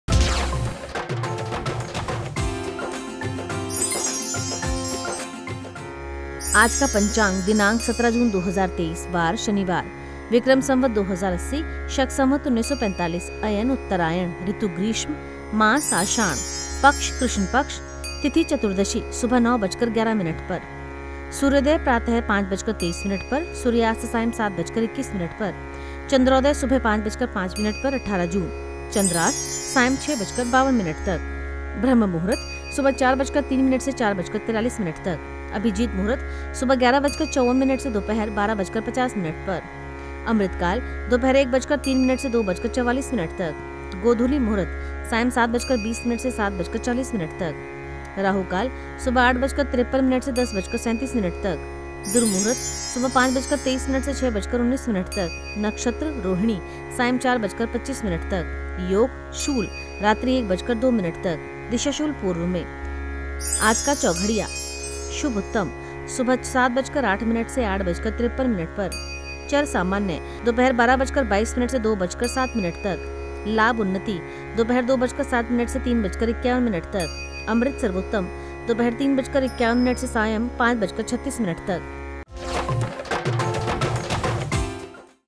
Aaj Ka Panchang | 17 जून 2023 | जानें आज के शुभ मुहूर्त और राहुकाल का समय | आषाढ़ कृष्ण पक्ष
सनातन पंचांग को वैदिक पंचांग के नाम से जाना जाता है। पंचांग के माध्यम से समय एवं काल की सटीक गणना की जाती है। पंचांग, मुख्य रूप से पांच अंगों से मिलकर बना होता है। ये पांच अंग तिथि, नक्षत्र, वार, योग और करण हैं। यहां, हम, दैनिक पंचांग में आपको शुभ मुहूर्त, राहुकाल, सूर्योदय और सूर्यास्त का समय, तिथि, करण, नक्षत्र, सूर्य और चंद्र ग्रह की स्थिति, हिंदूमास एवं पक्ष आदि की जानकारी देते हैं। आइए जानते हैं आज के शुभ मुहूर्त और राहुकाल का समय।